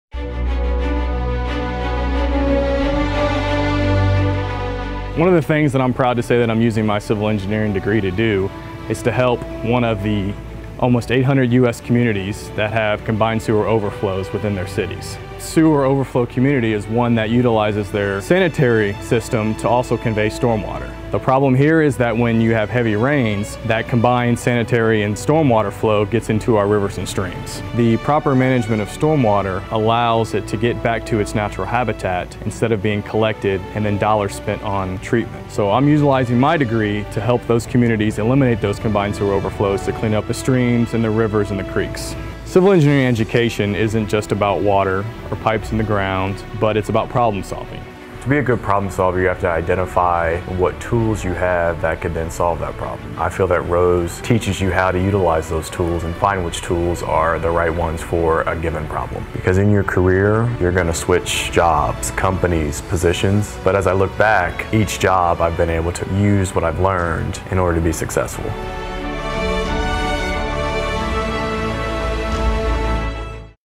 Civil & Environmental Engineering: Creating Safe Water | Rose-Hulman Institute of Technology
Providing safe treatment of wastewater is a challenge for communities around the world, even in the U.S.  Rose civil engineering alumnus, Maceo Lewis, talks about using his degree to solve the combined sewer overflow problem, resulting in less expensive wastewater treatment and higher quality waterways.